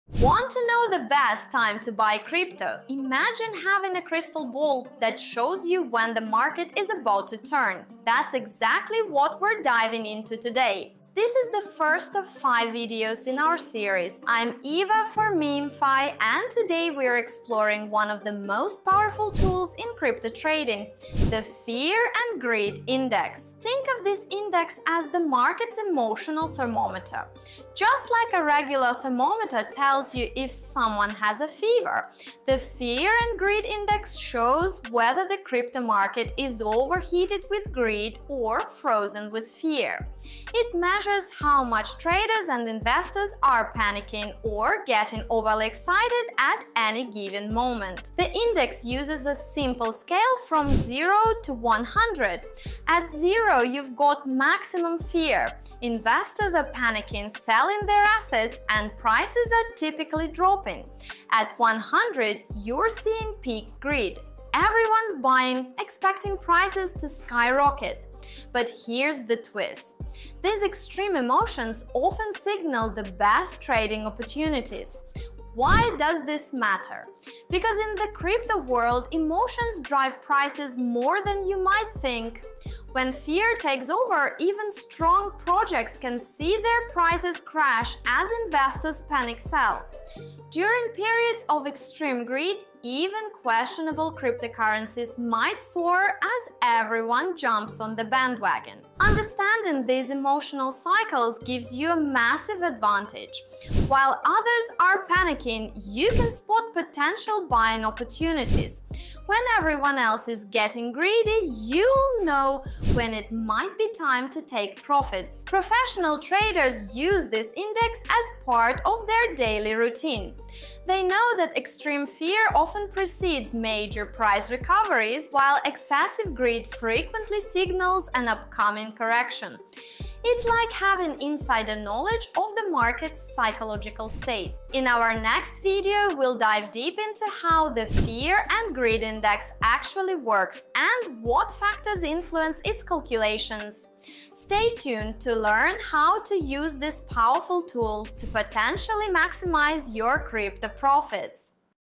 Fear & Greed: The Secret Crypto Trading Tool Explained! | Part 1 of 5 | MeemFi
The Fear and Greed Index reveals the crypto market’s emotional state, guiding traders to make smarter decisions. In this series, we explore how the index works, what data influences it, and how it can be used for strategies like contrarian trading, dollar-cost averaging, and momentum riding. From extreme fear to excessive greed, understanding this tool helps identify potential buy and sell points. Learn how professional traders interpret market sentiment to maximize profits and manage risks. Dive in to explore practical techniques for navigating market cycles with the Fear and Greed Index.